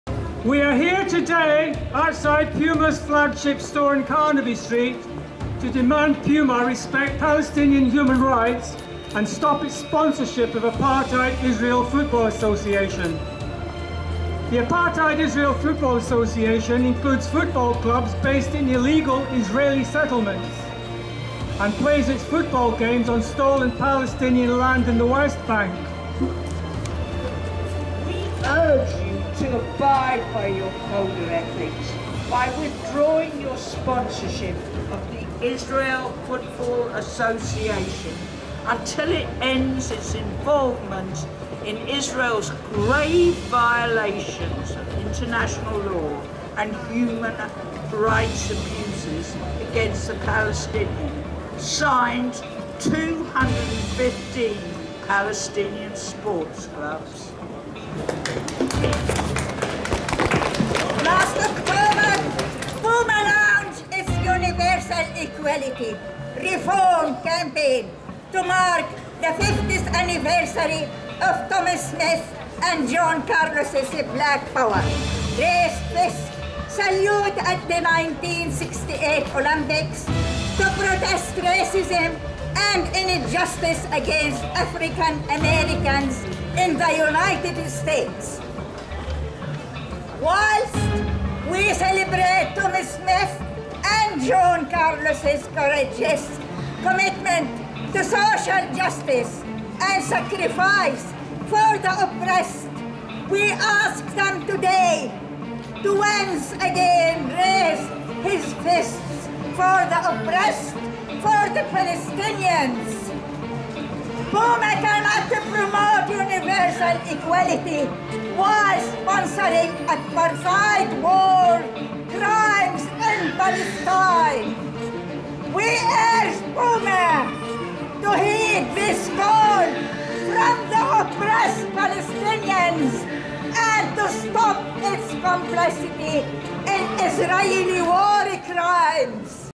London Protest Demands Puma End Complicity In Apartheid War Crimes
On Friday 15th February 2019, Inminds Human Rights Group protested outside Puma's London flagship store on Carnaby Street to demand Puma stop its complicity in Israel's war crimes by it sponsorship of apartheid Israel Football Association, which includes football clubs based in illegal Israeli settlements, and which plays its football games on stolen Palestinian land in the West Bank.

The protest was in solidarity with over 215 Palestinian sports clubs which on 10th September 2018 called on the world’s third largest sportswear manufacturer, Puma to respect Palestinian human rights and cut ties with the Israel Football Association.

Inminds chair Abbas Ali said "Last year we protested outside Adidas flagship store in London, in solidarity with 130 Palestinian sports clubs who had requested Adidas to stop supporting apartheid war crimes. After sustaining much brand damage Adidas stopped. Rather than learn from that experience, Puma has instead taken over from Adidas and signed a 4 year contract to lend its brand to cover up and whitewash the very same war crimes, which the illegal settlements constitute under international law. Illegal settlements built on stolen Palestinian land are normalised and legitimised by Puma through its sponsorship. This time 215 Palestinian sports clubs have asked Puma to respect their human rights. So far Puma has just ignored them. Sports is about fair play, Puma through its apartheid sponsorship is promoting racism.

"Last October Puma launched its 'universal equality' REFORM campaign to mark the 50th anniversary of Tommie Smith and John Carlos’s black power, raised fist, salute at the 1968 Olympics, to protest racism and injustice against African-Americans in the United States. Whilst we celebrate Tommie Smith and John Carlos’s courageous commitment to social justice, and sacrifice for the oppressed, we ask them today to once again raise his fists for the oppressed - for the Palestinians. Puma cannot promote 'universal equality' whilst sponsoring apartheid war crimes in Palestine. We urge Puma to heed this call from the oppressed Palestinians and to stop its complicity in Israeli war crimes!

"We are here to tell Puma that there is a price associated with standing with apartheid. Their complicity in Israeli war crimes will cost their brand dearly. We are here to ask Londoners to help Puma make the right decision by boycotting their products until they respect Palestinian human rights. "